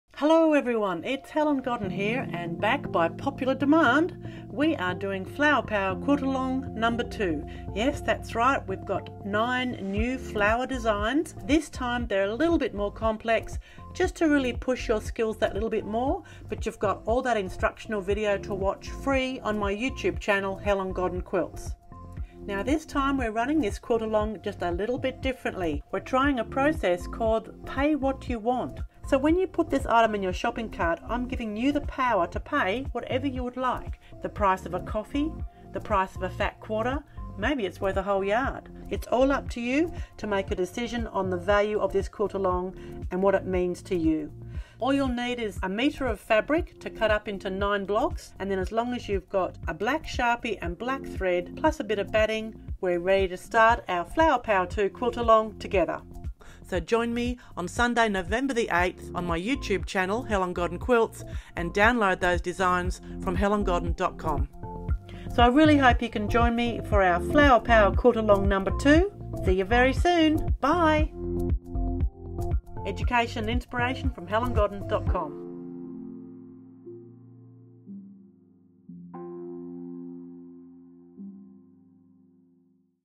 Flower Power 2 Quilt-a-Long! FREE QUILTING TUTORIAL
Helen Godden presents "Flower Power Quilt-a-long 2"!
Join in the free fun and learn to quilt 9 new designs to create a beautiful 9-patch quilt. Download the 9 new Flower Power designs from my website and prepare some fabric squares, black thread and Sharpie. Simple!

"Flower Power 2 Quilt-a-long" videos start on Sunday November 8th, and I will release a new instructional video every second day here on my YouTube channel 'Helen Godden Quilts'.

But this Quilt-a-long is a little different. I am trying a new 'Pay What You Want' approach. This means you can purchase the designs from my website, HelenGodden.com for whatever price you like... from zero to $$ dollars... Perhaps the price of a coffee? A fat quarter? A yard of fabric? It's totally up to you! Here is a link to try my "Pay What You Want" purchase.

Once you have purchased the pattern from my website, download and print out the designs to a consistent size. In my sample, the designs are 11” square, but you can print them any size you wish.

 I demonstrate all the quilting of every block, on my Handi Quilter Sweet Sixteen. You can quilt along with me on your domestic machine or even your long-arm machine.

You could decide to paint each block with Lumiere acrylic fabric paints, or work them with a pieced background and appliqué each flower. You may like to work these designs in embroidery or colour the designs with INKtense Pencils. Maybe you just want to print out the designs and doodle into them with a pen, to practise the quilting patterns I demonstrate.

Follow 'Helen Godden Quilts' on Facebook for more quilting Education and Inspiration.